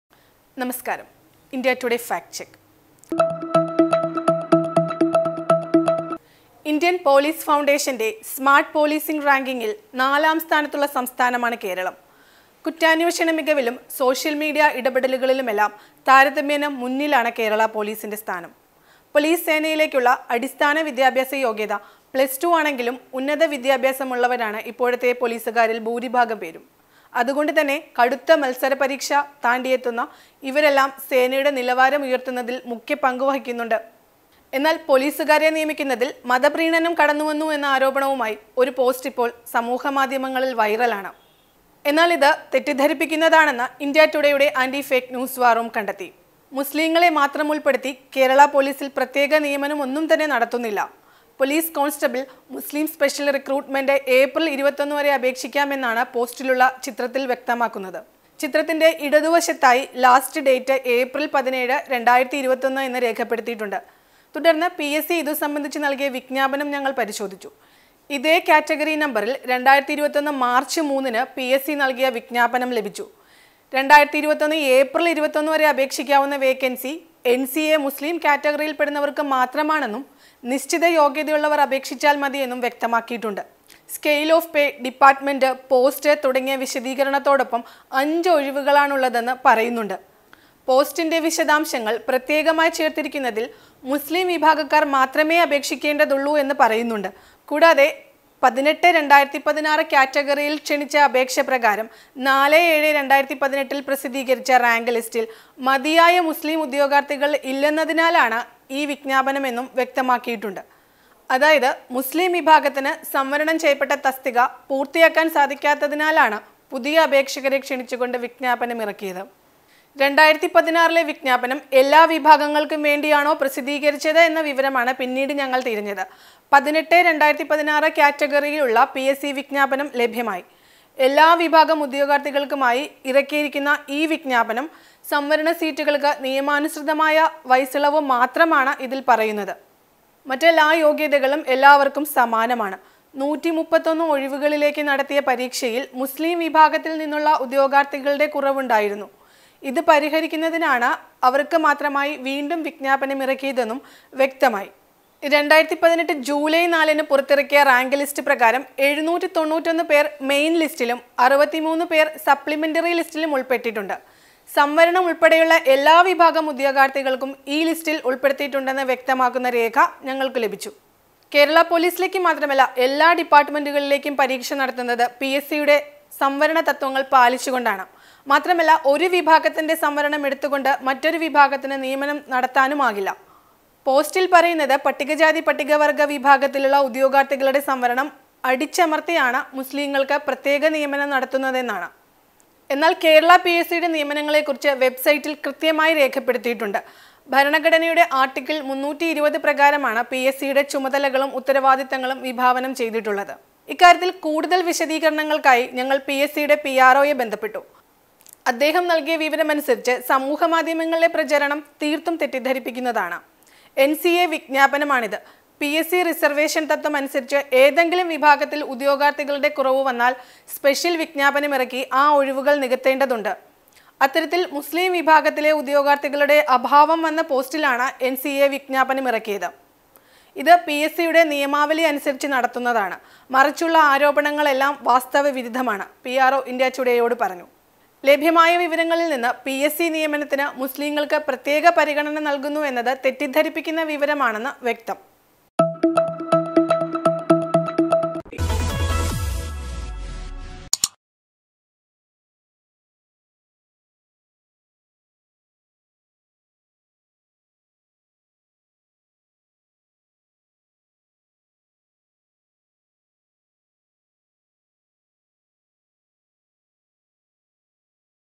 Fact Check: കേരള പൊലീസിലേക്ക് മുസ്ലീങ്ങളെ മാത്രം ഉള്‍പ്പെടുത്തി പ്രത്യേക നിയമനമോ ?
ഇന്ത്യന്‍ പൊലീസ് ഫൗണ്ടേഷന്റെ(ഐപിഎഫ്) സ്മാര്‍ട്ട് പൊലീസിംഗ് റാങ്കിംഗില്‍ നാലാംസ്ഥാനത്തുള്ള സംസ്ഥാനമാണ് കേരളം. കുറ്റാന്വേഷണ മികവിലും സോഷ്യല്‍ മീഡിയ ഇടപെടലുകളിലുമെല്ലാം താരതമ്യേന മുന്നിലാണ് കേരള പൊലീസിന്റെ സ്ഥാനം. പൊലീസ് സേനയിലേക്കുള്ള അടിസ്ഥാന വിദ്യാഭ്യാസ യോഗ്യത പ്ലസ്ടു ആണെങ്കിലും ഉന്നത വിദ്യാഭ്യാസമുള്ളവരാണ് ഇപ്പോഴത്തെ പൊലീസുകാരില്‍ ഭൂരിഭാഗം പേരും. അതുകൊണ്ട് തന്നെ കടുത്ത മത്സര പരീക്ഷ താണ്ടിയെത്തുന്ന ഇവരെല്ലാം സേനയുടെ നിലവാരം ഉയര്‍ത്തുന്നതില്‍ മുഖ്യപങ്കുവഹിക്കുന്നുണ്ട്. എന്നാല്‍ പൊലീസുകാരെ നിയമിക്കുന്നതില്‍ മതപ്രീണനം കടന്നുവന്നു എന്ന ആരോപണവുമായി ഒരു പോസ്റ്റ് ഇപ്പോള്‍ സമൂഹമാധ്യമങ്ങളില്‍ വൈറലാണ്.

' ഒരു മതേതര രാജ്യത്ത് ഇതു പോലെ ഒരു മതവിഭാഗത്തിന് മതത്തിന്റെ പേരില്‍ ജോലി കൊടുക്കുന്നത് ഒരിക്കലും അംഗീകരിക്കാനാകില്ല ഇത്തരം നടപടികള്‍ നമ്മുടെ ഭരണഘടന വിഭാവനം ചെയ്യുന്ന മതേതരതത്തിനു വിരുദ്ധമാണ് കമ്മ്യൂണിസ്റ്റ് സര്‍ക്കാര്‍ ഇതു പോലെ വര്‍ഗ്ഗീയമായ ചേരിതിരിവുകള്‍ സൃഷ്ടിച്ചു കേരളത്തെ ഇസ്ലാമികവത്കരിക്കുകയാണ് ' എന്നു തുടങ്ങുന്ന പോസ്റ്റിന്റെ പൂര്‍ണ്ണരൂപം